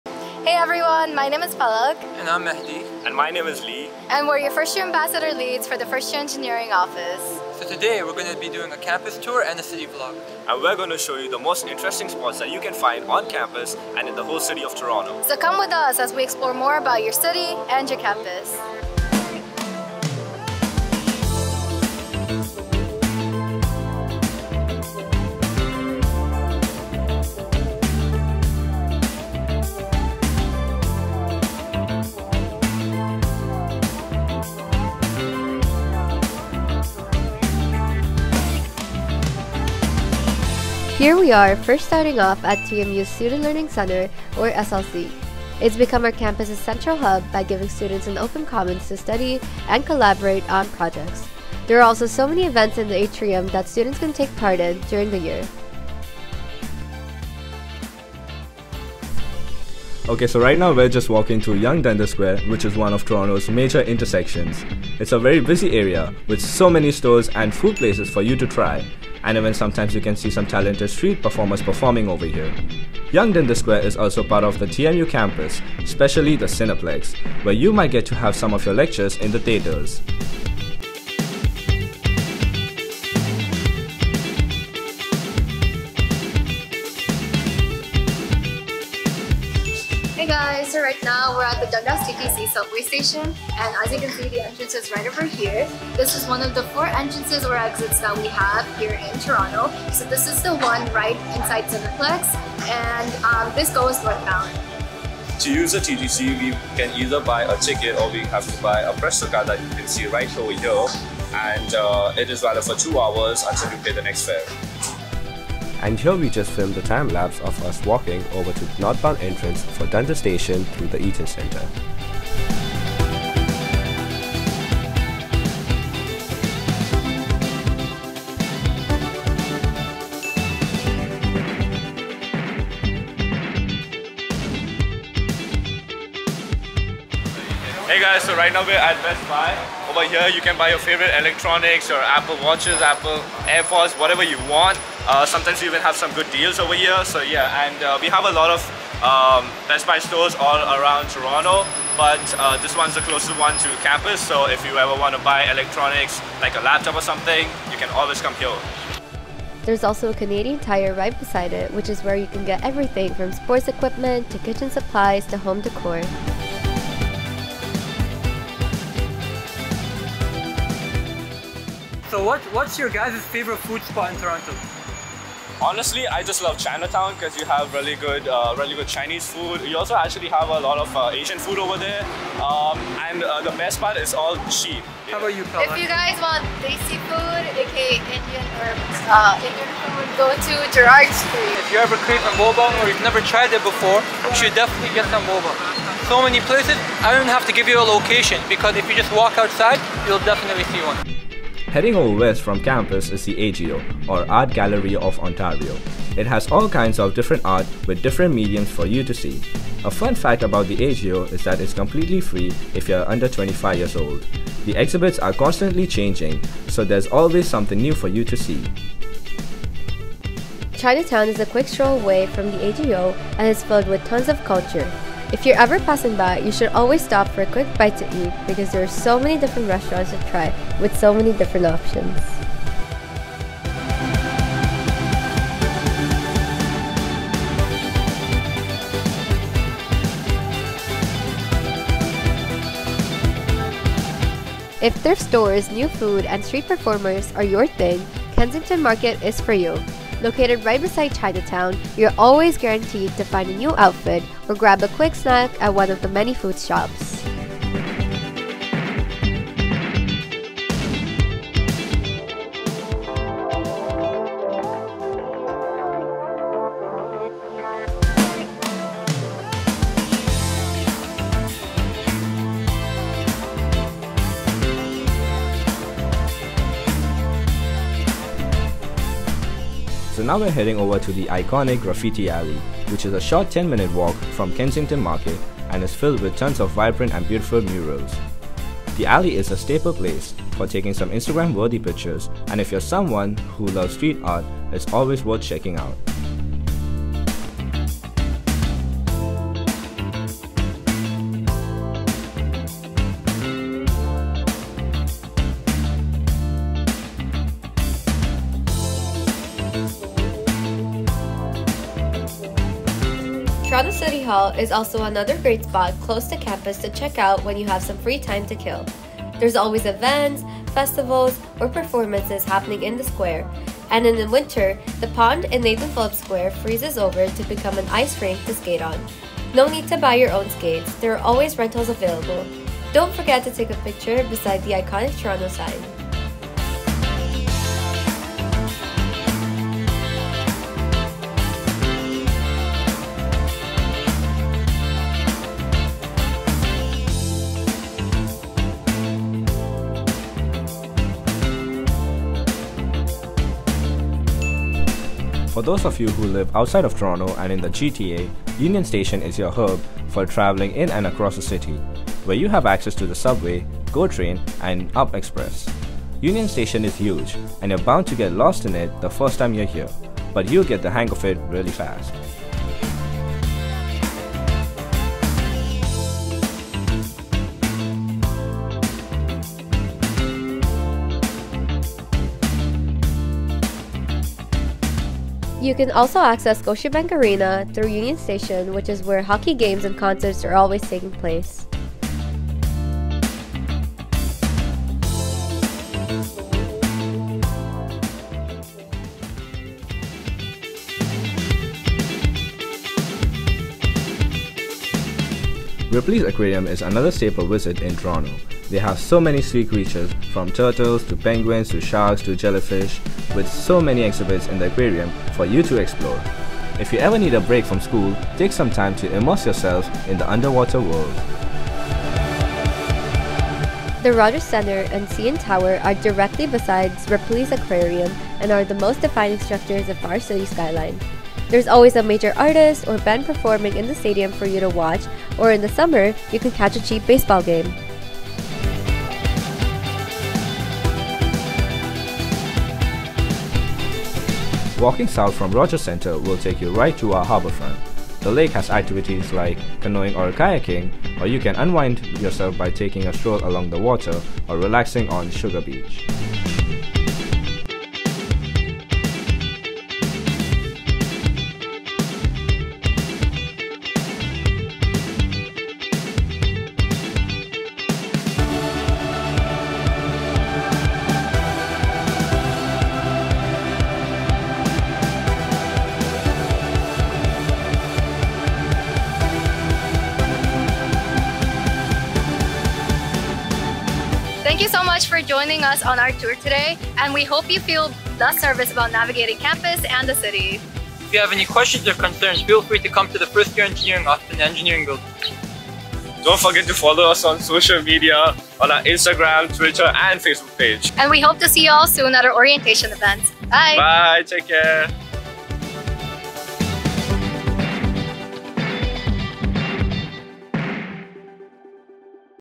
Toronto City Vlog - TMU Engineering
Attention all incoming Engineering students! Join Palak, Mahdi, and Li (your First-Year Ambassador Leads) as they take you on a tour around the city of Toronto! Learn about all the trendy spots you can visit, places you can eat, and things to do in the city while completing your studies!

Recorded and edited by: Lucas Moscati
Recorded by: Katie Ngo
Produced and starring: Palak Dedhia
Starring: Mahdi Hassen and Li Shu Ma